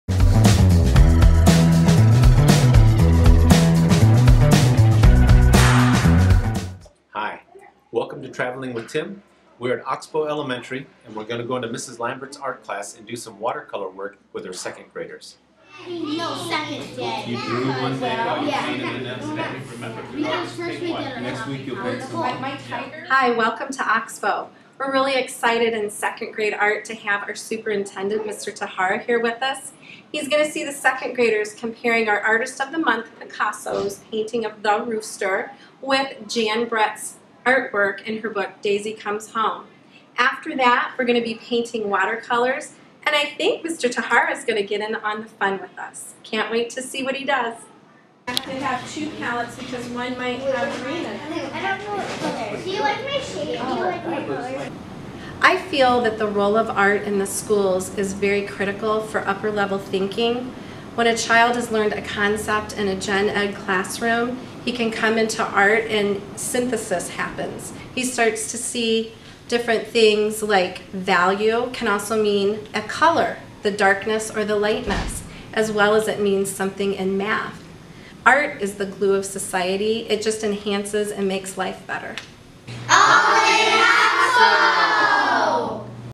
Traveling with Tim | Episode 1: "Art Adventures at Concord Ox Bow"
In this inaugural episode of "Traveling with Tim," Concord Community Schools Superintendent Tim Tahara visits Valerie Lambert's art class at Concord Ox Bow Elementary School.